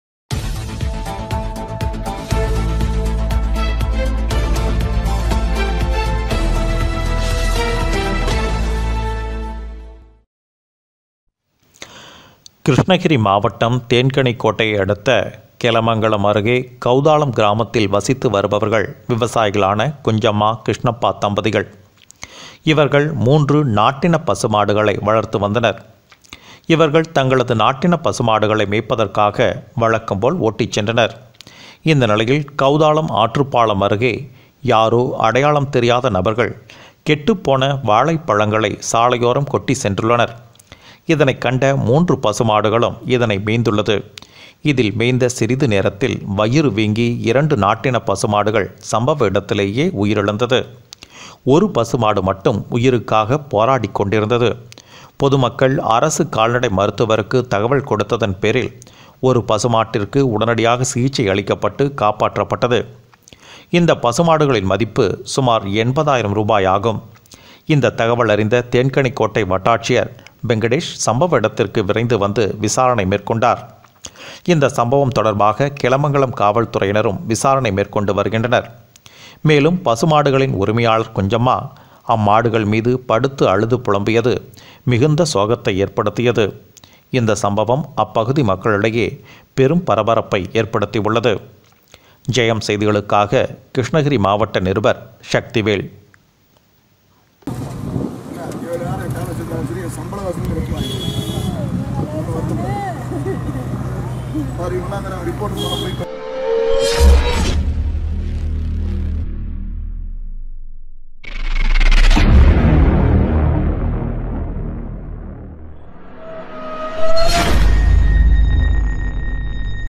தேன்கனிக்கோட்டை அருகே அழுகிய வாழைப்பழம் சாப்பிட்ட இரண்டு நாட்டின பசு மாடுகள் பலி
கிருஷ்ணகிரி மாவட்டம் தேன்கனிக்கோட்டை அடுத்த கெலமங்கலம் அருகே கெளதாளம் கிராமத்தில் வசித்து வருபவர் விவசாயிகளான குஜ்ஜம்மா, கிருஷ்ணப்பா தம்பதிகள்  இவர்கள் மூன்று  நாட்டின  பசு மாடுகளை வளர்த்து வந்தனர்.

இவர்கள் தங்களது நாட்டின பசுமாடுகளை மேய்ப்பதற்காக வழக்கம் போல் ஓட்டிச் சென்றனர், இந்நிலையில் கெளதாளம் ஆற்று பாலம் அருகே யாரோ அடையாளம் தெரியாத நபர்கள்  கெட்டு போன வாழைப்பழத்தை சாலையோரம் கொட்டி சென்றுள்ளனர். இதனை கண்ட மூன்று பசு மாடுகளும்  இதனை மேய்ந்துள்ளது, இதில்  மேய்ந்த சிறிது நேரத்தில் வயிறு வீங்கி இரண்டு நாட்டின பசு மாடுகள் சம்பவ இடத்திலேயே உயிர் இறந்து விட்டது.

ஒரு பசு மாடு மட்டும் உயிருக்காக போராடிக் கொண்டிருந்தது, பொதுமக்கள் அரசு கால்நடை மருத்துவருக்கு தகவல் கொடுத்ததின் பேரில் ஒரு பசு மாட்டிற்கு உடனடி சிகிச்சை அளிக்கப்பட்டு காப்பாற்றப்பட்டது. இந்த பசு மாடுகளின் மதிப்பு  80 ஆயிரம் ஆகும் இந்த தகவல் அறிந்த தேன்கனிக்கோட்டை வட்டாட்சியர் வெங்கடேஷ் சம்பவம் நடந்த இடத்திற்கு விரைந்து வந்து விசாரணை நடத்தினார்.

இந்த சம்பவம் தொடர்பாக கெலமங்கலம் காவல்துறையினரும் விசாரணை மேற்கொண்டு வருகின்றனர். மேலும் பசு மாடுகளின் உரிமையாளர் குஜ்ஜம்மா அம்மாடுகள் மீது படுத்து அழுது புலம்பியது  இந்த சம்பவம் அப்பகுதி மக்களிடையே பெரும் பரபரப்பை ஏற்படுத்தி உள்ளது.

ஜெயம் செய்திகளுக்காக கிருஷ்ணகிரி மாவட்டம் நிருபர் சக்திவேல்